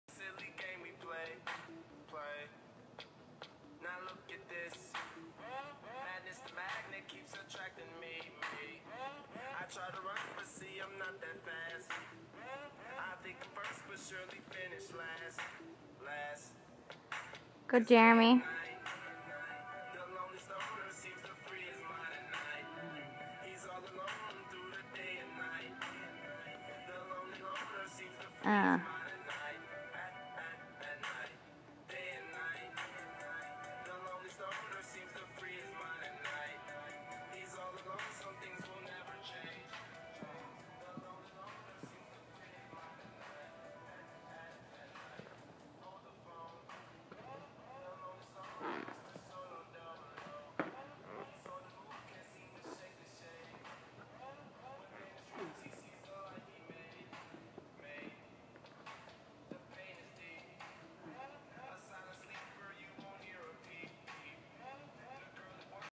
Artist Jeremy Adams painting mural Stewart Eagles
Painting to music at a gym mural I did for Stewart Middle School. Painting highlights on the feathers and blending shadows.